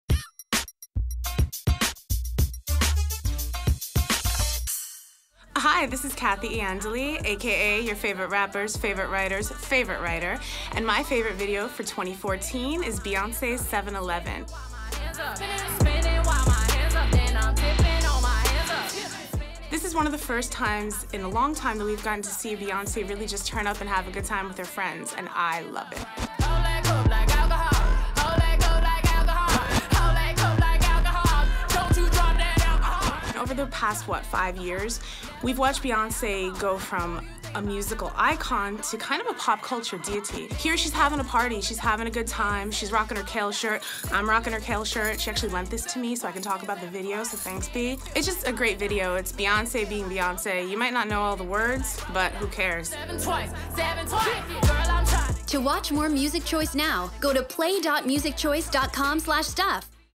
Kathy's Fave Music Video of 2014
Music writer Kathy Iandoli's KALE shirt can be seen in her favorite music video of 2014. Any guesses?

For more of the biggest artists of the year, be sure to check out the MC 100: Top 25 on MC On Demand - MC 100!